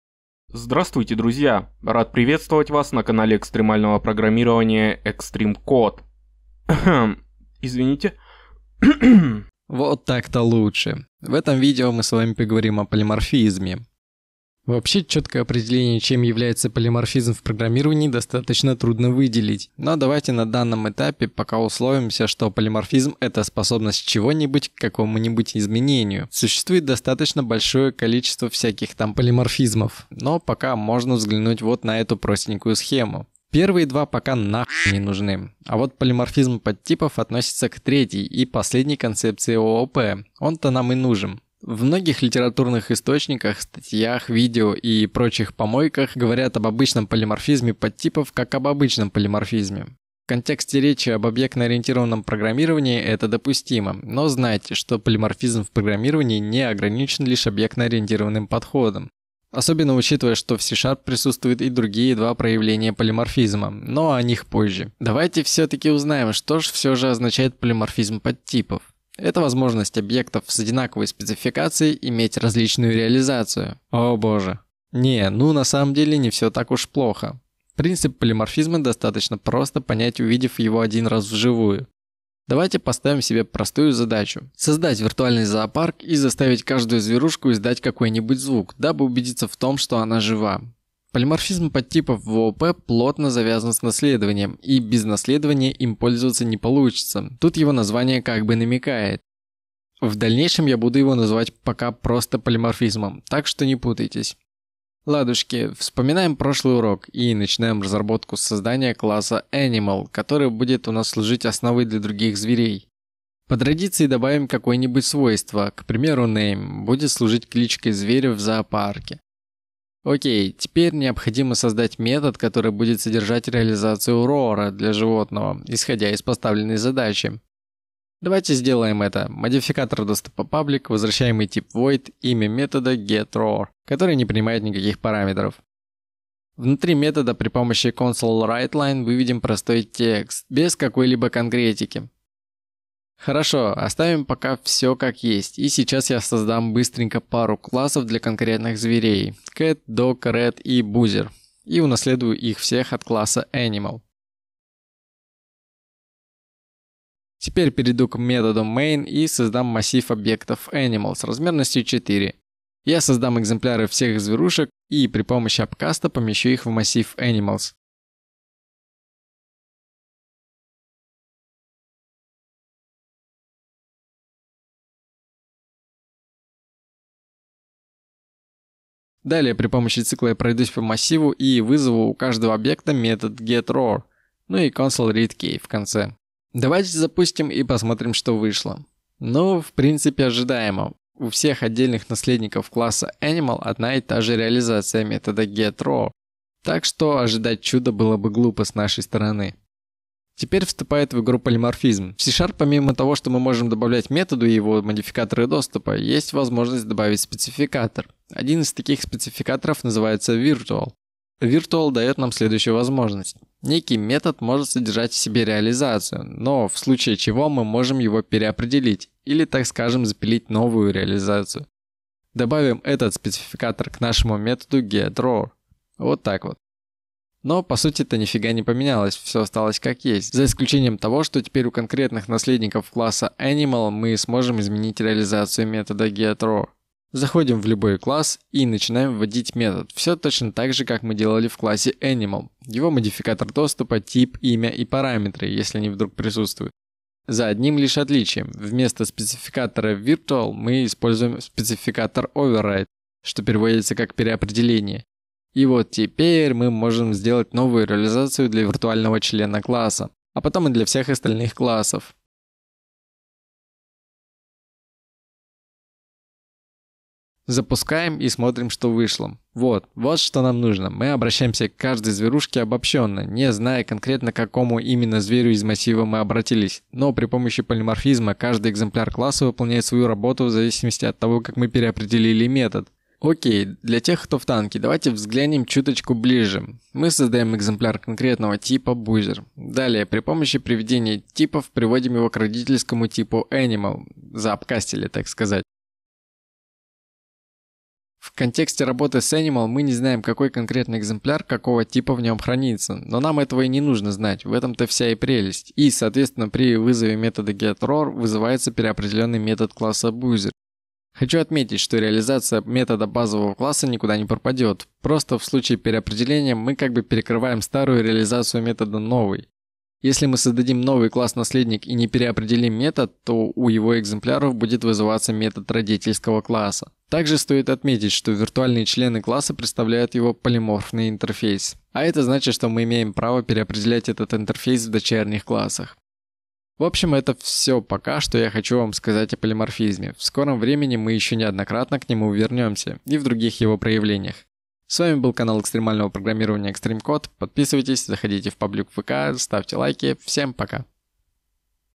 C# - Полиморфизм. Уроки для маленьких и тупых #13.
Хэй, продолжаются наши уроки C# для маленьких и тупых. В этом выпуске мы познакомимся с полиморфизмом